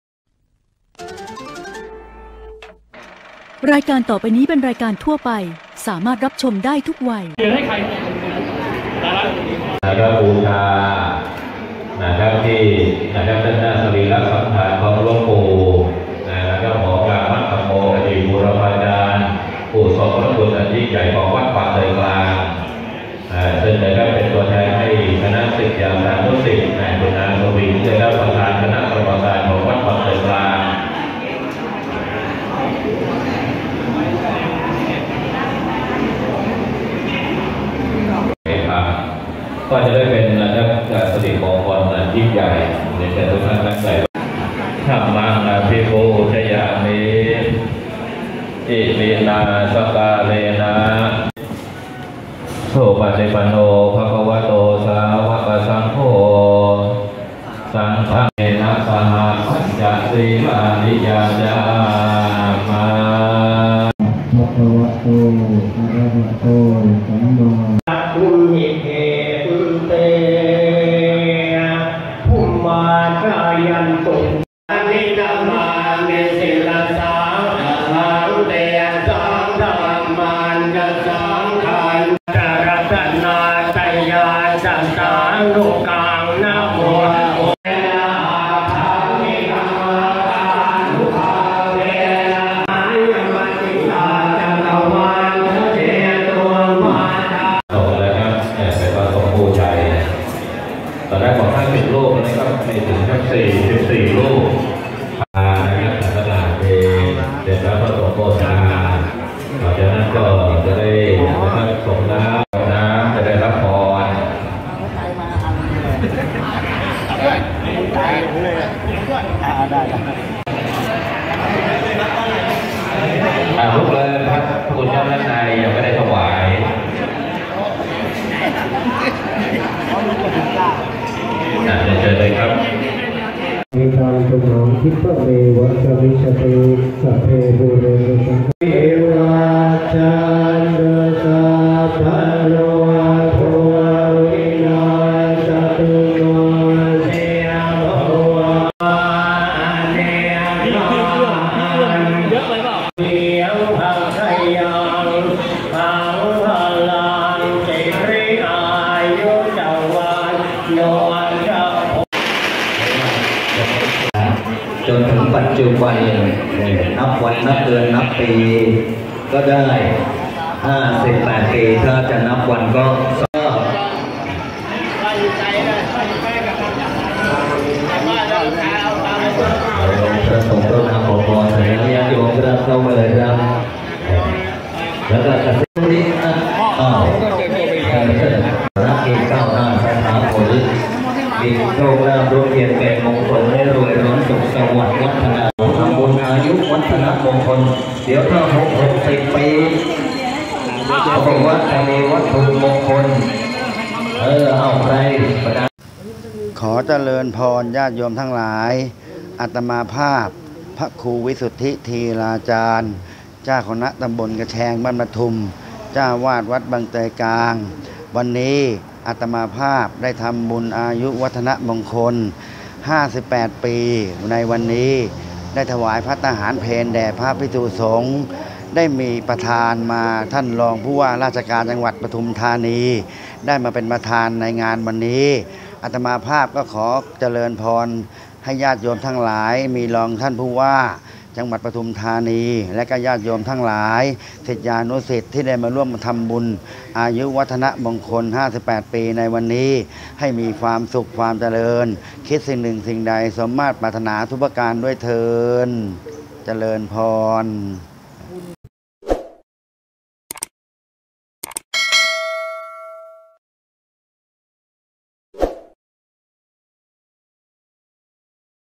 ปทุมธานี รองผู้ว่าราชการจังหวัดปทุมธานี เป็นประธานทำบุญครบรอบอายุวัฒนมงคล ๕๘ ปี พระครูวิสุทธิธีราจาร
ปทุมธานี รองผู้ว่าราชการจังหวัดปทุมธานี เป็นประธานทำบุญครบรอบอายุวัฒนมงคล ๕๘ ปี พระครูวิสุทธิธีราจาร(พระอาจารย์อนงค์)คณะตำบลกระแชง-บ้านปทุม เจ้าอาวาสวัดบางเตยกลาง กันอย่างคึกคัก

วันที่ ๒๙ มิถุนายน ๒๕๖๗ ผู้สื่อข่าวรายงานจากวัดบางเตยกลาง ตำบลบางเตย อำเภอสามโคก จังหวัดปทุมธานี นายคมสัน ญาณวัฒนา
รองผู้ว่าราชการจังหวัดปทุมธานี ทำบุญครบรอบอายุวัฒนมงคล ๕๘ ปี พระครูวิสุทธิธีราจาร (พระอาจารย์อนงค์) เจ้าคณะตำบลกระแชง-บ้านปทุม เจ้าอาวาสวัดบางเตยกลาง
พร้อมด้วยศิษยานุศิษย์พี่น้องประชาชน เข้าร่วมพิธีเป็นจำนวนมาก ส่วนบรรยากาศภายในงานมีศิษยานุศิษย์มาออกโรงทานขนมคาวหวาน ก๋วยเตี๋ยว เพื่อให้ผู้ที่มาร่วมทำบุญได้รับประทาน(ฟรี) และก่อนเริ่มพิธีได้มีศิษยานุศิษย์ ทั้งใกล้และไกลต่างทยอยกันเดินทางมาที่วัดบางเตยกลาง ซึ่งเป็นสถานที่จัดงาน  ต่อมาเมื่อเวลา ๑๐.๐๐ น.นายคมสัน ญาณวัฒนา รองผู้ว่าราชการจังหวัดปทุมธานี ให้เกียรติเป็นประธาน (ประธานฝ่ายฆราวาส)ได้เดินทางมาถึงจากสถานที่จัดงาน  ต่อมาประธานได้จุดธูปเทียนบูชาพระรัตนตรัย ต่อมาพระสงฆ์ทรงสมณศักดิ์ เจริญพระพุทธมนต์ ๙ รูป ต่อมา เมื่อเวลา ๑๑.๐๐ น.พุทธศาสนิกชนศิษย์ยานุศิษย์ถวายภัตตาหารเพลแด่พระภิกษุสงฆ์และสามเณร ๔๗ รูป ต่อมาพระครูสุทธิธีรจารย์ (พระอาจารย์อนงค์)เจ้าคณะตำบลกระแชง-บ้านปทุม เจ้าอาวาสวัดบางเตยกลาง จัดพิธีน้อมถวายมุทิตาจิตสักการะอายุวัฒนมงคล มีพระสงฆ์ในอำเภอสามโคกจังหวัดปทุมธานีพร้อมด้วยศิษย์ยานุศิษย์ ร่วมพิธีส่งน้ำอวยพร จากนั้นท่านพระครูสุทธิธีราจารย์ ได้มอบวัตถุมงคล (หลวงปู่หอม)ให้ศิษยานุศิษย์ ที่มาร่วมงาน เพื่อเป็นศิริมงคลแก่ตนเองและครอบครัวสืบไป

สหรัฐ แก้วตา หน.ข่าว ปทุมธานี รายงาน